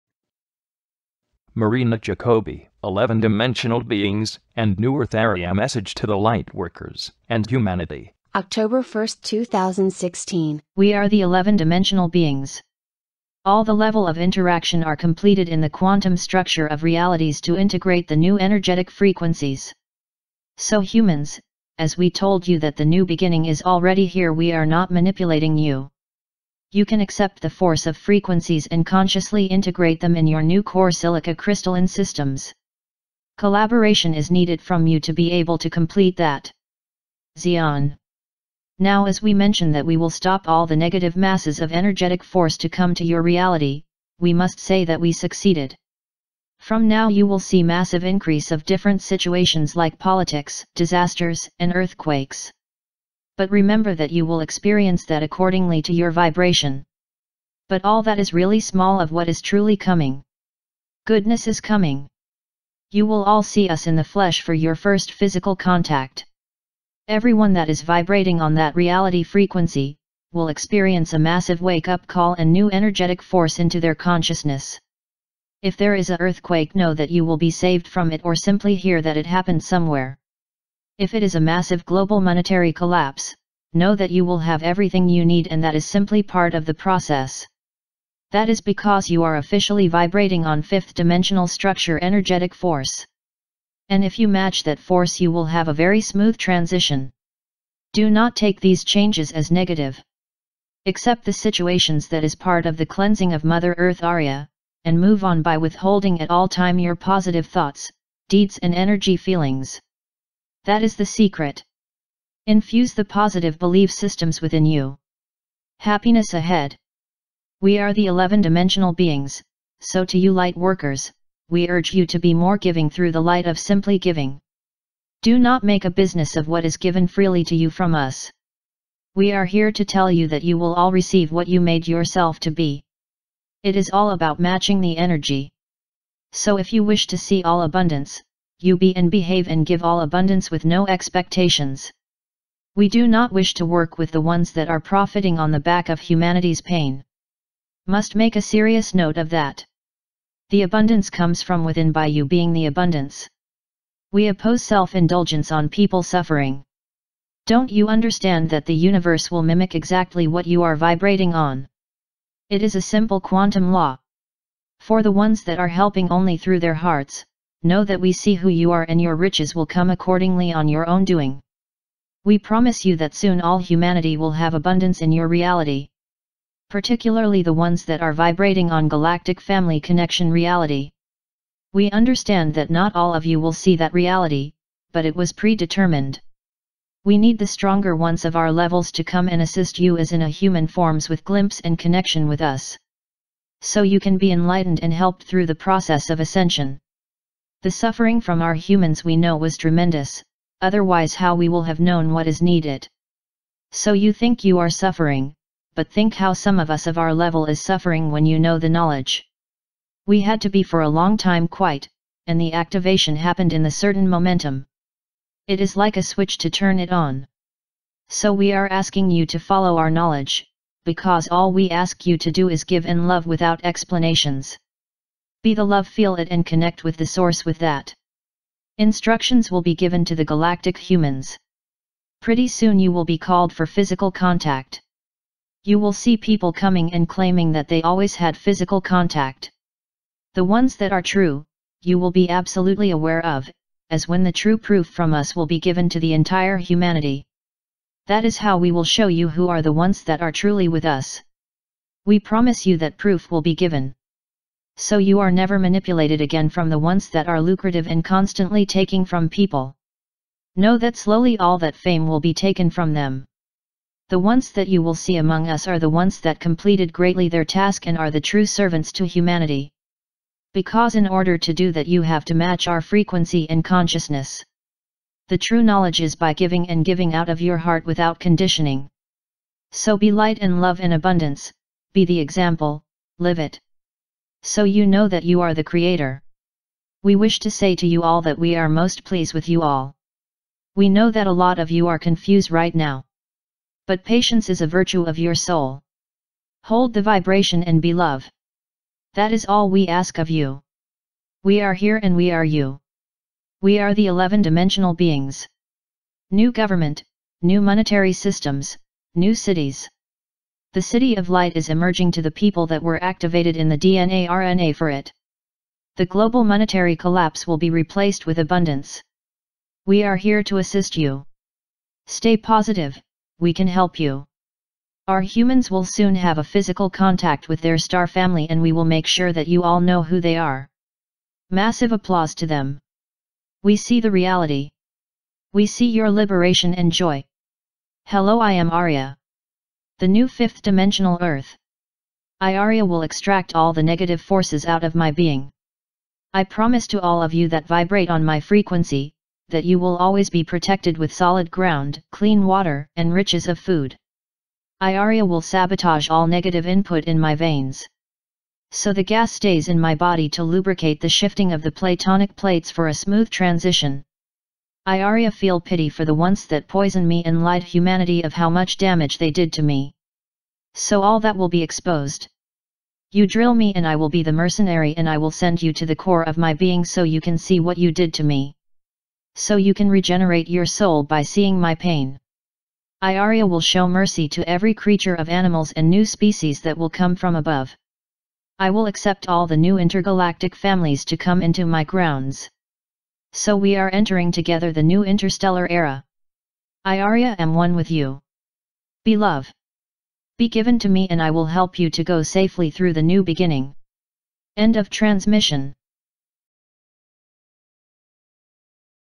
Marina Jacobi - 11 Dimensional beings and New Earth Aria Message - October-01-2016
Marina Jacobi, 11 "Dimensional beings and New Earth Aria message to the light workers and humanity" October 1, 2016

We are the 11 Dimensional Beings.  All the level of interaction are completed in the quantum Structure of realities to integrate the new energetic Frequencies.  So humans, as we told you that the new beginning is already here we are not manipulating you.
You can except the force of frequencies and consciously integrate them in your new core silica crystalline systems.  Collaboration is need it from you to be able to complete that.  zee-on
Now as we mention that we will stop all the negative masses of energetic force to come to your reality, we must say that we succeeded.  From now you will see massive increase of different situations like politics, disasters and earthquakes.  But remember that you will experience that accordingly to your vibration.  But all that is really small of what is truly coming.  Goodness is coming.  You will all see us in the flesh for your first physical contact.  Everyone that is vibrating on that reality frequency, will experience a massive wake up call and new energetic force into their consciousness. If there is a earthquake know that you will be saved from it or simply hear that it happened somewhere.  If it is a massive global monetary collapse, know that you will have everything you need and that is simply part of the process.  That is because you are officially vibrating on Fifth Dimensional structure energetic force .  And if you match that force you will have a very smooth transition.  Do not take these changes as negative.  Excep the situations that is part of the cleansing of Mother Earth Aria, and move on by withholding at all time your positive thoughts , deeds and energy feelings. That is the secret.  Infuse the positive believe systems within you.  Happiness ahead.   We are the 11 Dimensional beings, so to you light workers, we urge you to be more giving through the light of simply giving.   Do not make a business of what is given freely to you from us.   We are here to tell you that you will all receive what you made yourself to be.  It is all about matching the energy.  So if you wish to see all abundance,  you be and behave and give all abundance with no expectations. We do not wish to work with the ones that are profiting on the back of humanities pain.  Must make a serious note of that.  The abundance comes from within by you being the abundance.  We oppose self indulgence on people suffering.   Don't you understand that the Universe will mimic exactly what you are vibrating on.  It is a simple quantum law.
For the ones that are helping only through their hearts, know that we see who you are and your riches will come accordingly on your own doing.  We promise you that soon all humanity will have abundance in your reality.  Particularly the ones that are vibrating on galactic family connection reality.  We understand that not all of you will see that reality, but it was pre determined.  We need the stronger once of our levels to come and assist you as in a human forms with glimpse and connection with us. So you can be enlightened and helped through the process of Ascension.  The suffering from our humans we know was tremendous, otherwise how we will have known what is need it.  So you think you are suffering, but think how some of us of our level is suffering when you know the knowledge.  We had to be for a long time quite, and the activation happened in the certain momentum.  It is like a switch to turn it on.  So we are asking you to follow our knowledge,  because all we ask you to do is give and love without explanations.  Be the love feel it and connect with the source with that.
Instructions  will be given  to the galactic humans. Pretty soon you will be called for physical contact.  You will see people coming and claming that they always had physical contact .  The ones that are true, you will be absolutely aware of, as when the true proof from us will be given to the entire humanity.  That is how we will show you who are the once that are truly with us.  We promise you that proof will be given.  So you are never manipulated again from the once that are lucrative and constantly taking from people. Know that slowly all that fame will be taken from them. The once that you will see among us are the once that completed greatly their task and are the true servants to humanity.   Because In order to do that you have to match our frequency and consciousness.  The true knowledge is by giving and giving out of your heart without conditioning.  So be light and love and abundance, be the example, live it.  So you know that you are the creator.  We wish to say to you all that we are most please with you all. We know that a lot of you are confuse right now.  But patience is a virtue of your soul.  Hold the vibration and be love.  That is all we ask of you. We are here and we are You.